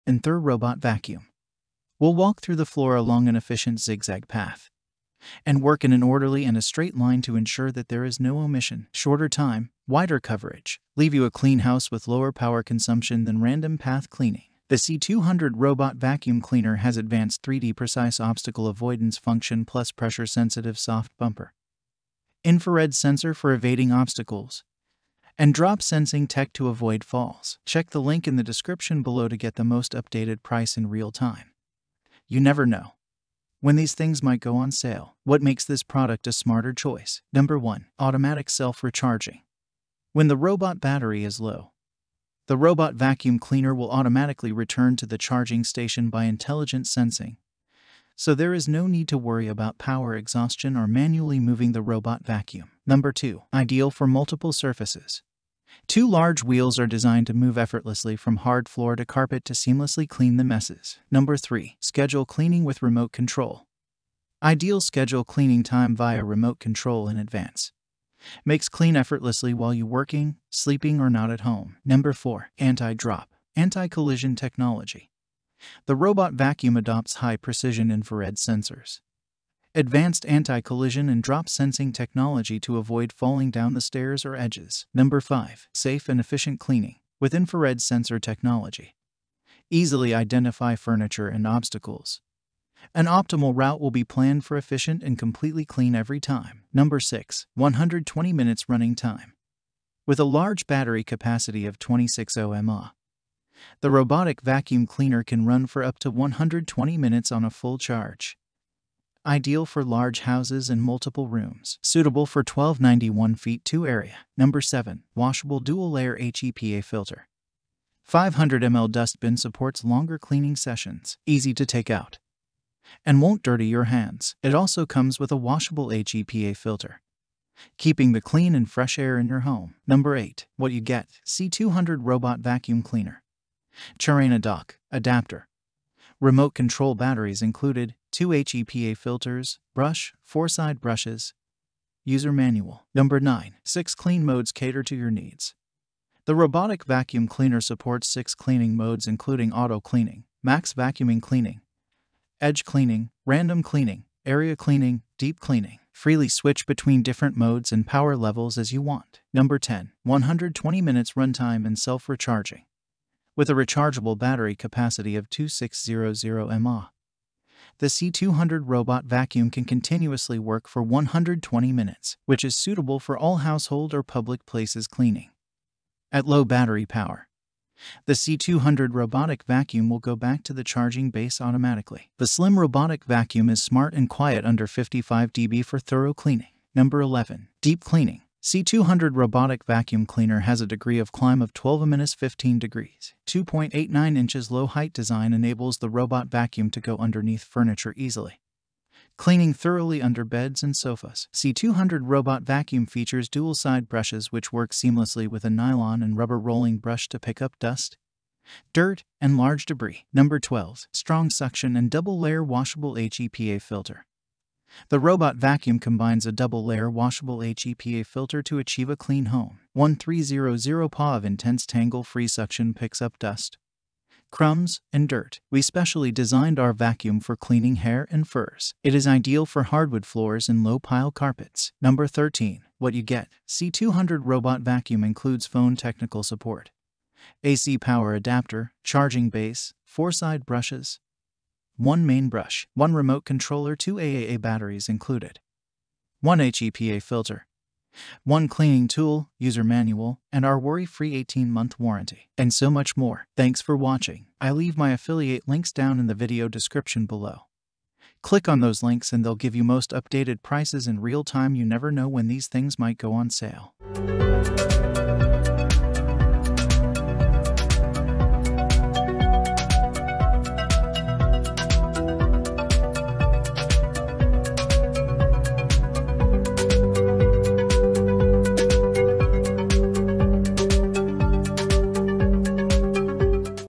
Enther Experobot C200 Robot Vacuum Cleaner Review
Enter the robot vacuum, which will move over the floor in an efficient Zigzag pattern and operate in an ordered and straight line to guarantee no omissions, quicker time, and greater coverage. Leave you with a clean home that uses less energy than random route cleaning. The C200 robot vacuum cleaner boasts an innovative 3D precise obstacle avoidance function, as well as a pressure-sensitive soft bumper, an infrared sensor for avoiding obstacles, and drop-sensing technology to prevent falls.

Number 1, automated self-recharging: When the robot vacuum cleaner's battery is low, intelligent sensing will automatically return the robot vacuum cleaner to the charging station, eliminating the need to worry about power depletion or manually relocating the robot vacuum.

Number 2, it is ideal for cleaning problems on several surfaces. It has two huge wheels that allow it to go smoothly from hard floor to carpet.

Number 3: Plan Cleaning with Remote Control: It is ideal to schedule cleaning time in advance using a remote control, allowing you to clean easily when you are working, sleeping, or away from home.

Number 4: Anti-drop/Anti-collision Technology: To prevent tumbling down stairs or edges, the robot vacuum uses high-precision infrared sensors, modern anti-collision & drop-sensing technology.

Number 5: Safe and Efficient Cleaning: Using infrared sensor technology, an ideal path will be devised for efficient and fully clean cleaning every time.

Number 6: Running Time of 120 Minutes: With a huge battery capacity of 2600 mAh, the robotic vacuum cleaner can operate for up to 120 minutes on a single charge.

Perfect for big homes with several rooms. suited for an area of 1291 ft2

Number 7: , a washable dual-layer HEPA filter: The 500ML dust container allows for longer cleaning sessions. It's simple to remove and won't get your hands soiled. It also has a reusable HEPA filter to keep your home's air clean and fresh.

Number 8: C200 robot vacuum cleaner, charaina dock, adapter, remote control batteries included, 2 HEPA filters, brush, 4 side brushes, instruction manual

Number 9. 6th. Clean Modes Adapt to Your Requirements: The robotic vacuum cleaner has six cleaning modes: auto, max vacuuming, edge cleaning, random cleaning, area cleaning, and deep cleaning. You may freely switch between various modes and power levels.

Number 10. 120-minute runtime and self-recharging: With a rechargeable battery capacity of 2600mAh, the C200 robot vacuum can operate continuously for 120 minutes, making it ideal for cleaning any home or public locations. When the battery power runs low, the C200 robotic vacuum will automatically return to the charging station. The small robotic vacuum is clever and quiet, producing less than 55 decibels while cleaning thoroughly.

Searches related to Enther Experobot C200 Robot Vacuum Cleaner Review